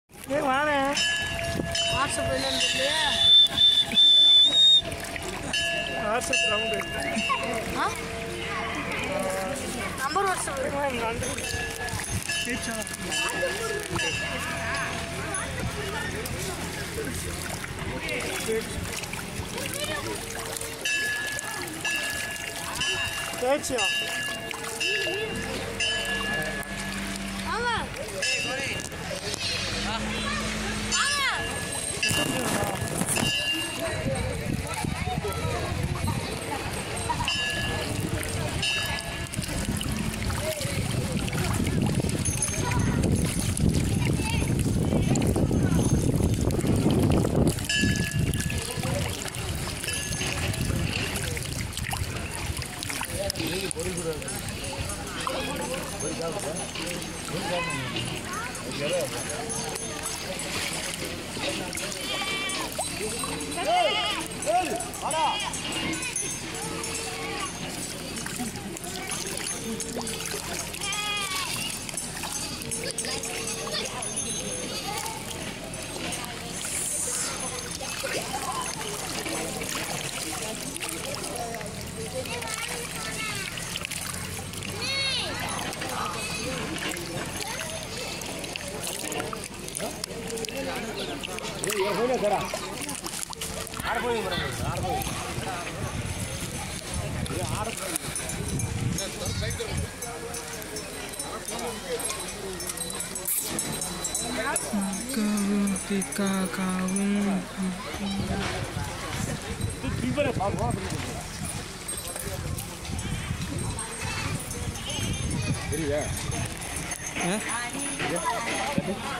Feeding to Fish | Sathuragiri | Madurai
Brother murugan's son ear piercing in mahalingam(thampipatti)

Place: Sathuragiri

Nearest city: Madurai